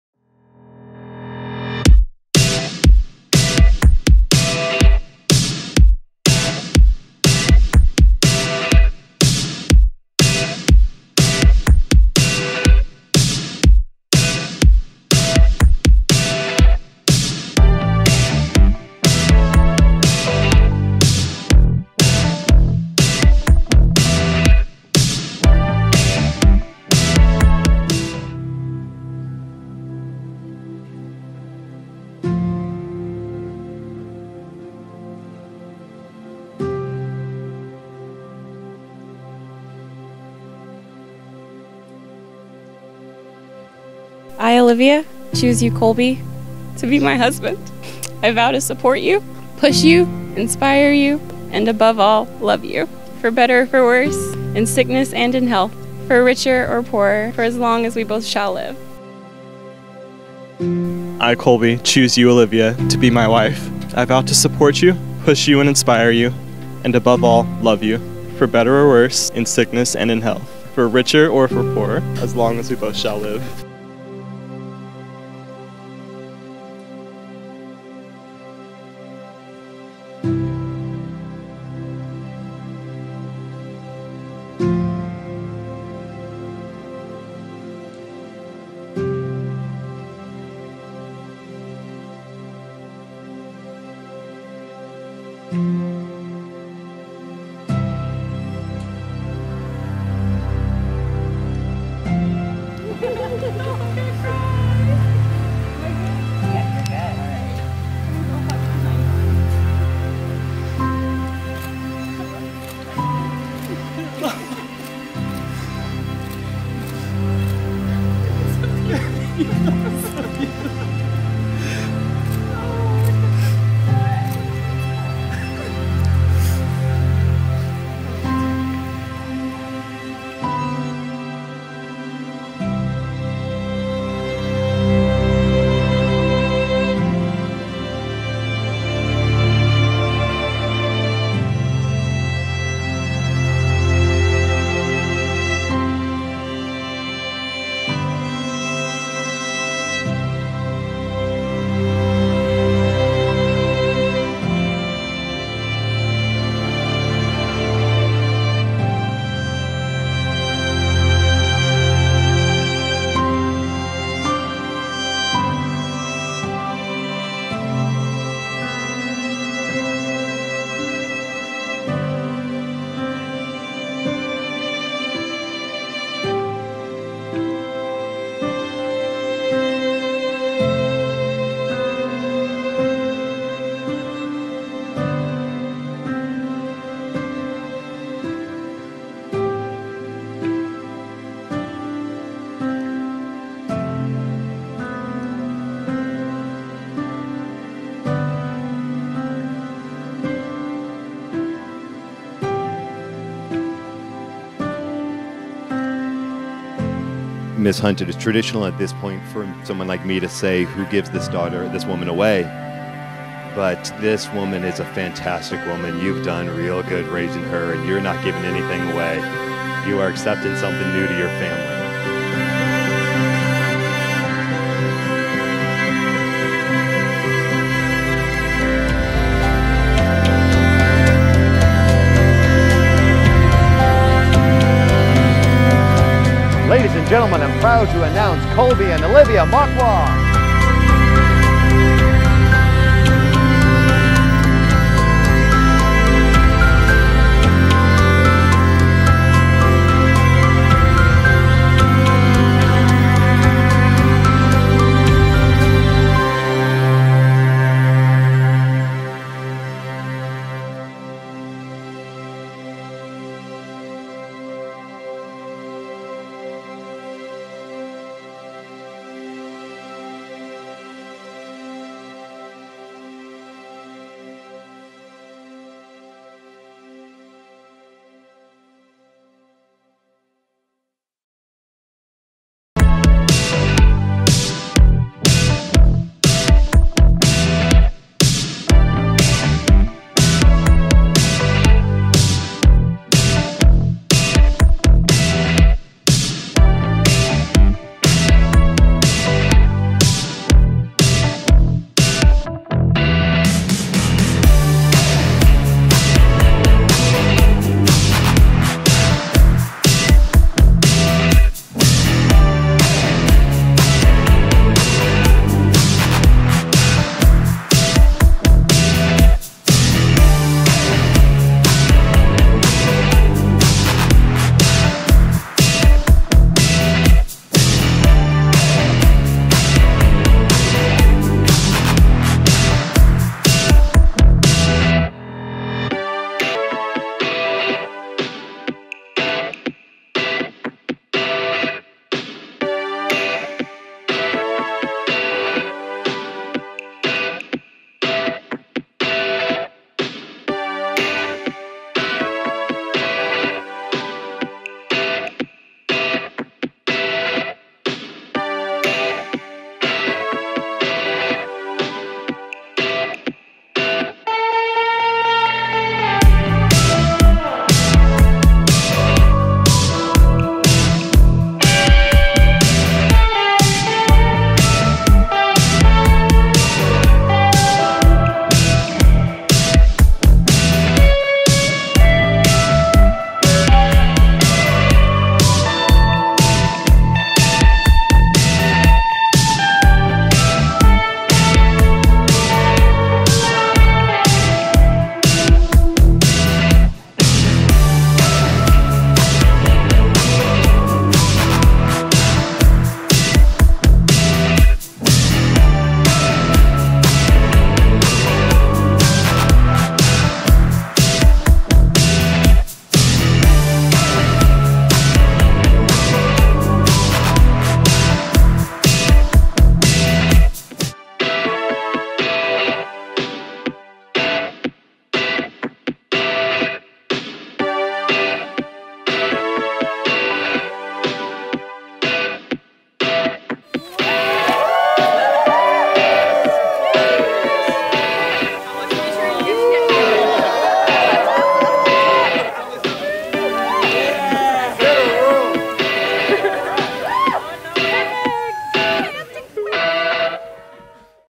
Groom cries when he sees his Bride - Greystone Estate
Check out this gorgeous wedding at Greystone Estate. Not only were Olivia and Colby one of the most down to earth couples I've ever had, they also knew how to have a blast. From including their dog in their wedding to doing New Orleans style wedding dancing, there was never a dull moment. Check out the film!